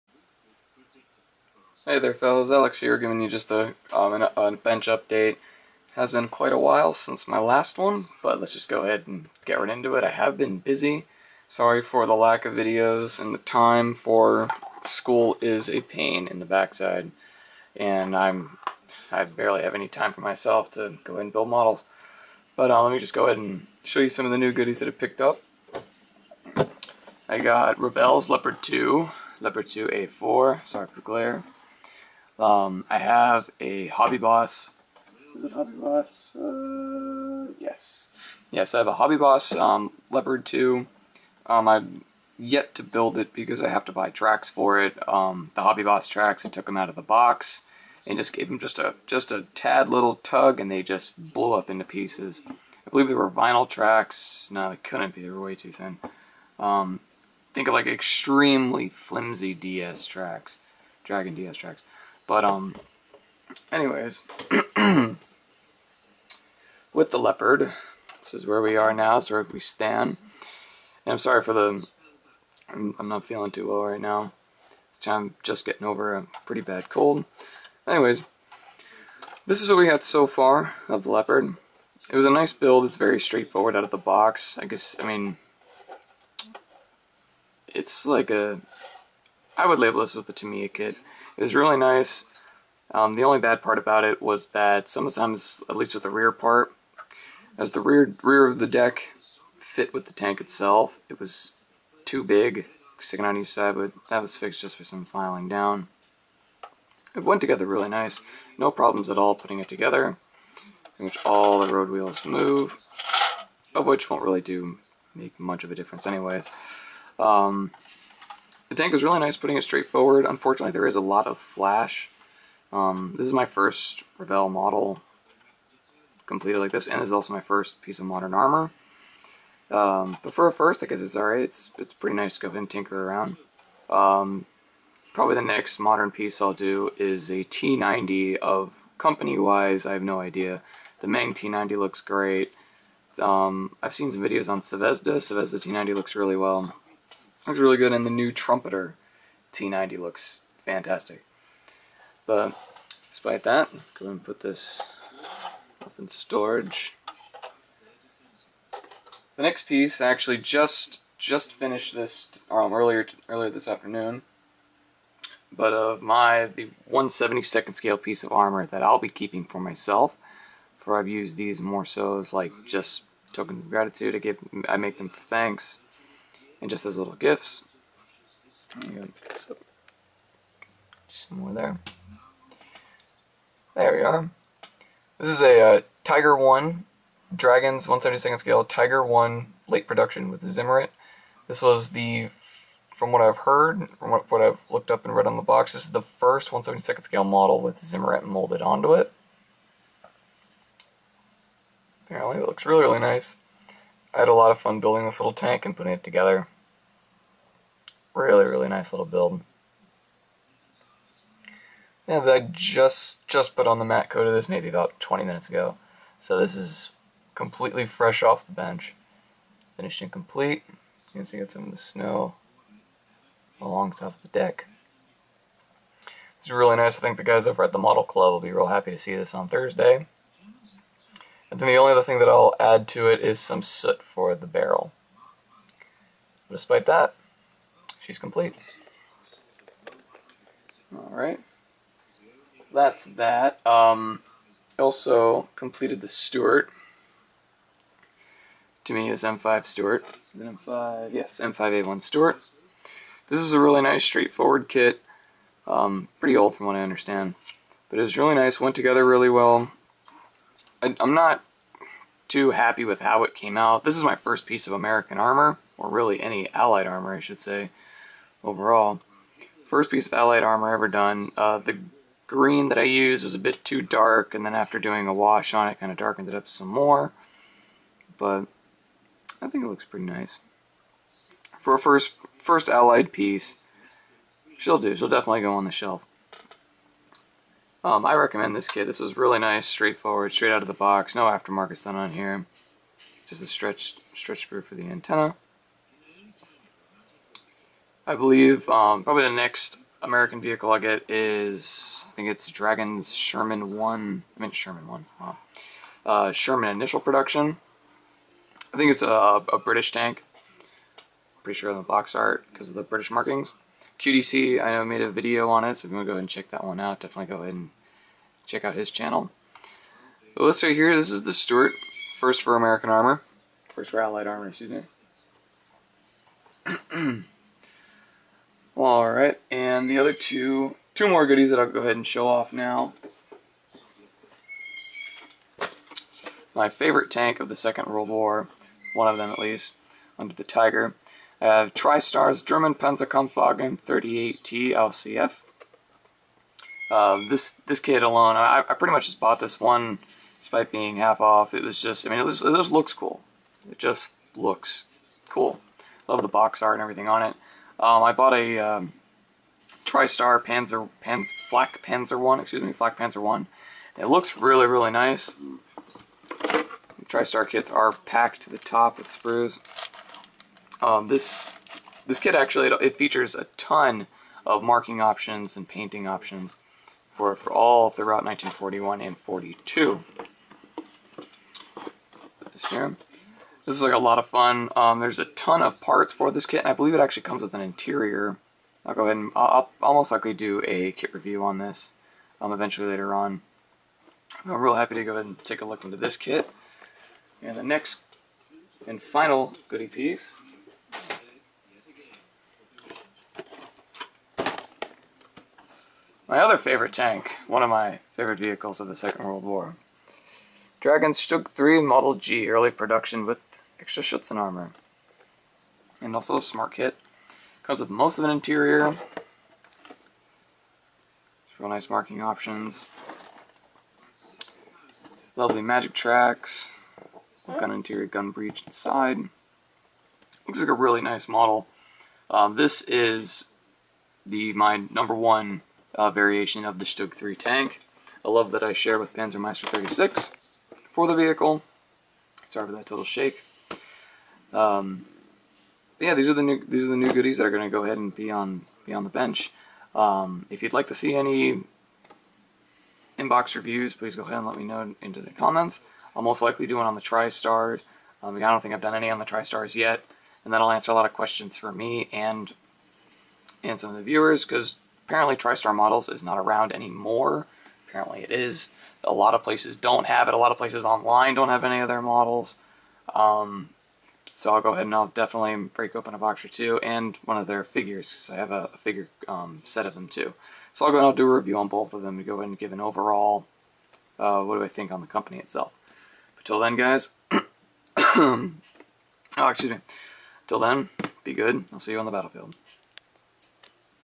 {Bench Update}: New builds! 1:72 Diorama
Just a quick update on the builds and my recently finish 1;72 Tiger I.